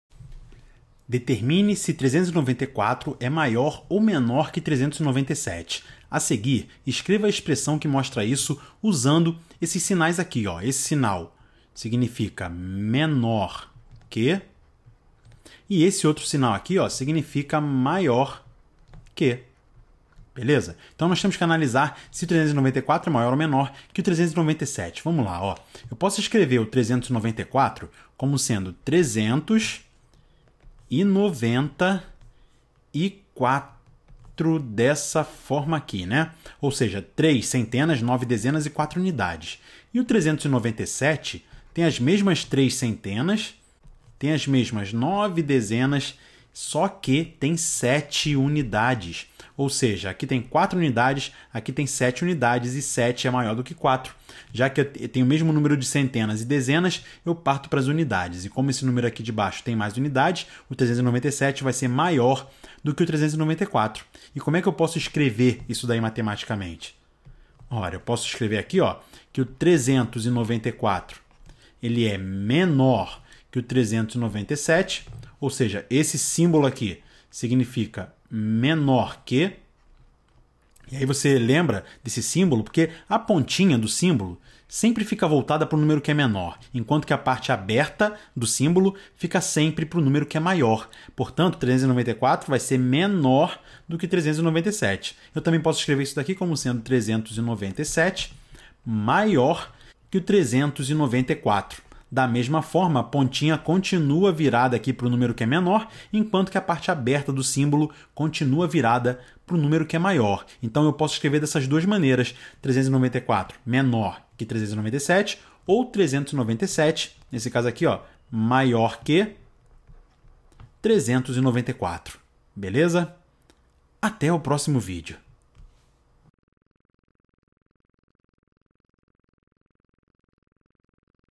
Comparando números inteiros
Entendendo a comparação entre dois números inteiros.
Titulo em inglês: Comparing whole numbers